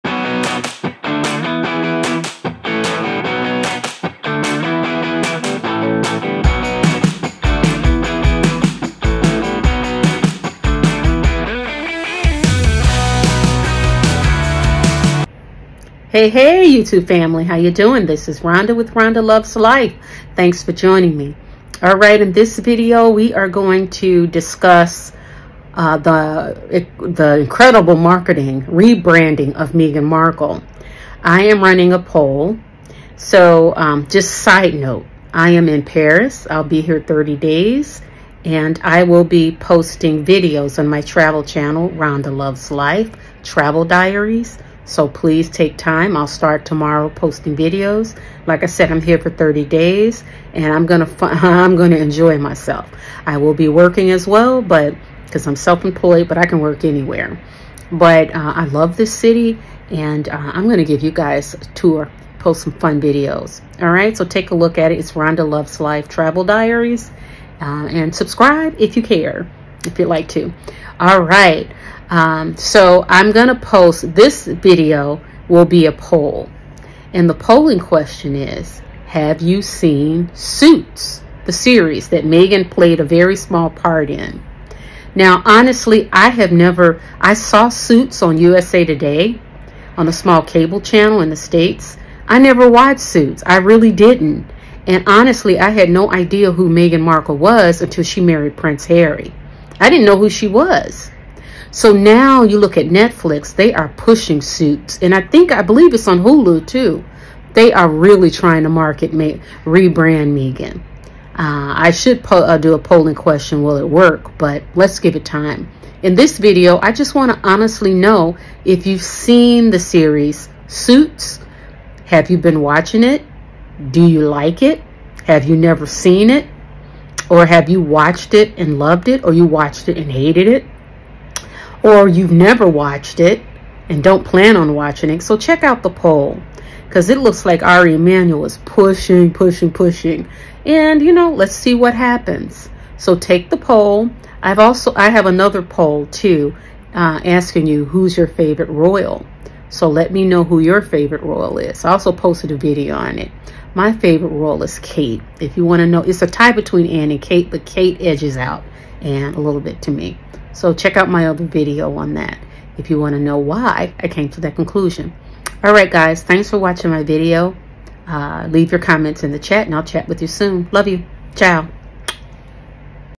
POLL: The Rebranding of Meghan Markle. Have You Seen “Suits”?
Ari Emanuel and WME are working overnight to rebrand Meghan Markle. One push is to market the old series “Suits”.  A cable series Meghan Markle had a tiny role in. The series was originally on the USA Network. Now it’s being pushed on Netflix AND Hulu. Must be nice to snag an idiot prince.

Well I can honestly say, I never watched the series “Suits” and never heard of Meghan Markle before she married Prince Harry.  Today, I still have no interest in seeing the show.

Have you seen Suits? Take the poll and let us know what you think.